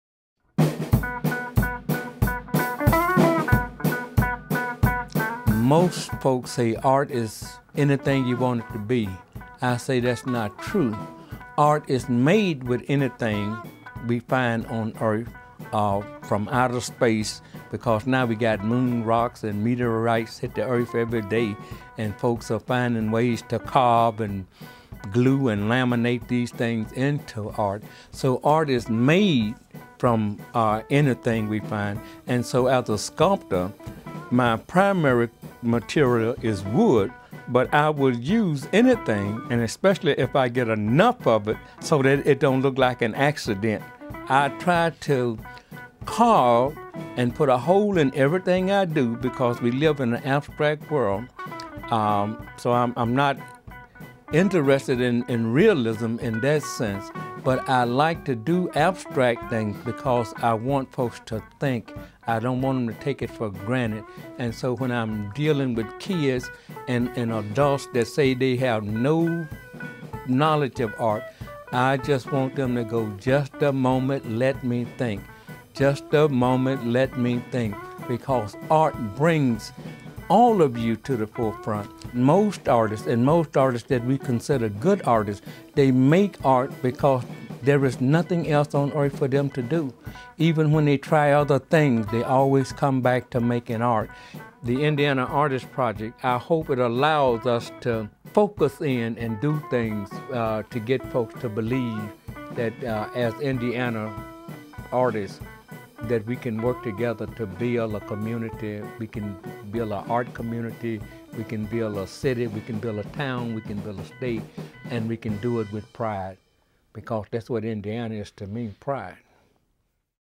Jake Webster - Wood & Stone Sculptures Indiana Artisan
Jake Webster has been carving since 1974 and painting for at least a decade prior to that. He travelled the world before putting down roots in northern Indiana more than 15 years ago.

Most artists I know come from the small town world. That gift doesnt change because we live at home, Jake says. In my case my art is enhanced by where I live. Indiana has some of the best limestone in the world. We have some of the best wood in the world.

Jakes finished carvings express the humanistic quality of people, their communities and the environment. He strives to create conversation, not just beautiful art. As a former art educator, Jake knows art helps people, especially kids, talk about things that are difficult. It gives us a way of communicating without pointing fingers or shaking our fists.

While he uses a traditional method of direct carving, Jake also adds recycled nails, bones and scrap metal to his sculptures and uses paper, rags, and product packages in his collage and paintings. When he can, he even uses found wood from fallen logs and scrap limestone from the quarries.

The wind provides the wood, Jake explains. Ive been recycling since before it was cool.

Jake gives back to his community by curating exhibits and holding art-related workshops for adults and children. His work also can be found in many private and public collections.